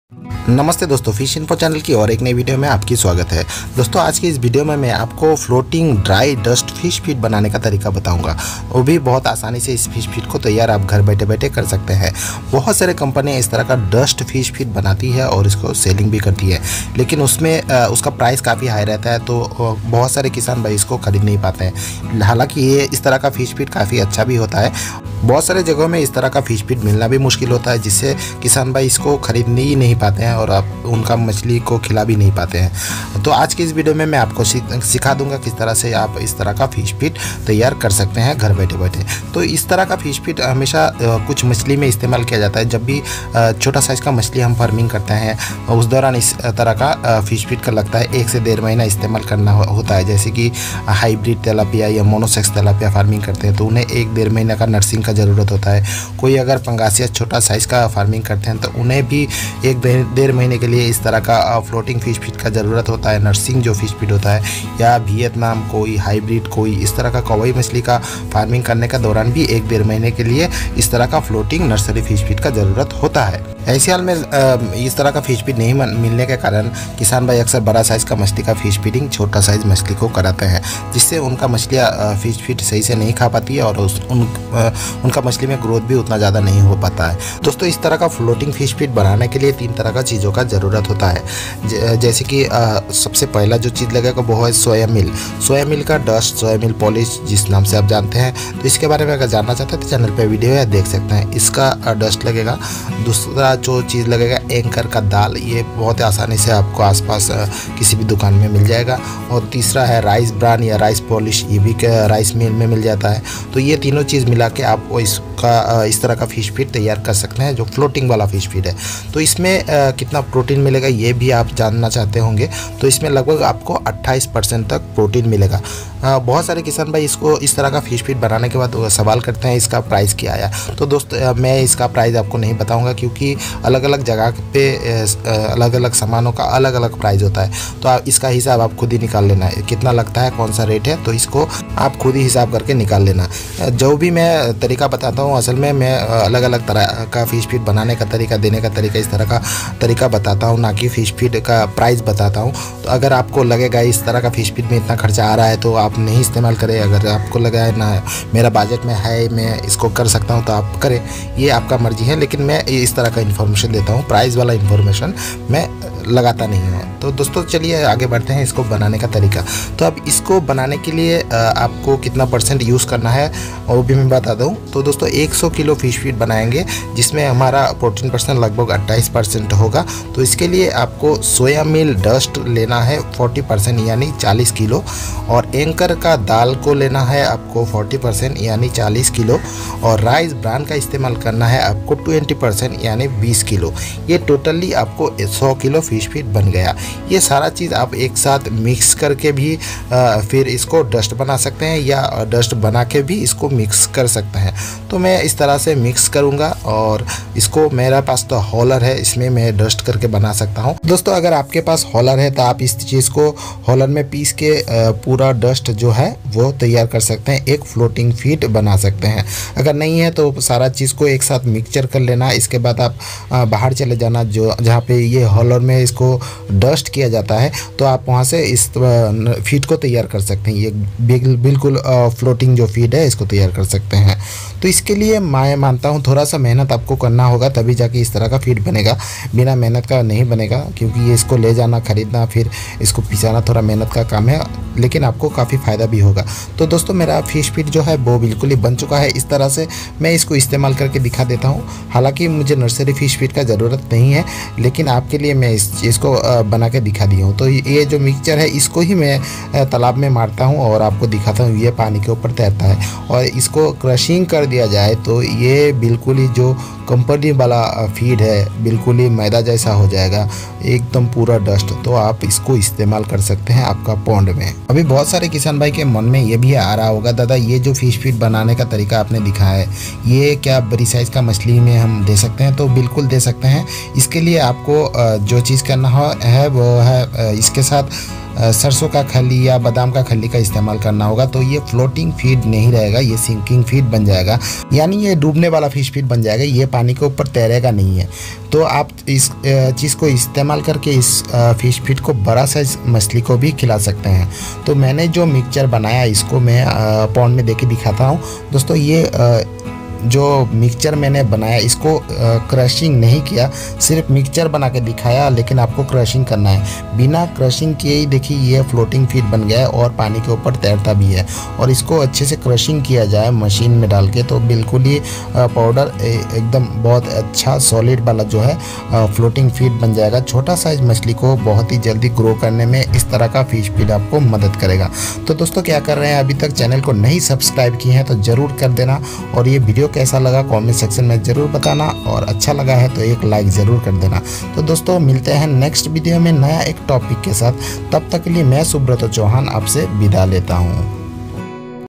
dust fish feed making process in hindi | dust floating powder fish feed making
video topic:- dust powder floating fish feed making full process at home, and dust powder making process with machine,fish feed kaise banaye

using ingredients:-
anchor dal powder, rice polish powder, soybean mile powder

 mixing process:-
Soybean meal 40%---40 kg
Anchor dal. 40%---40kg
Rice bran. 20%---20kg
Total.                 100%---100kg

Our project Name:- M/s Ganga Matshya hatchery
address:-Purbo Collegepara, (Jalkhana More)Raiganj, uttar Dinajpur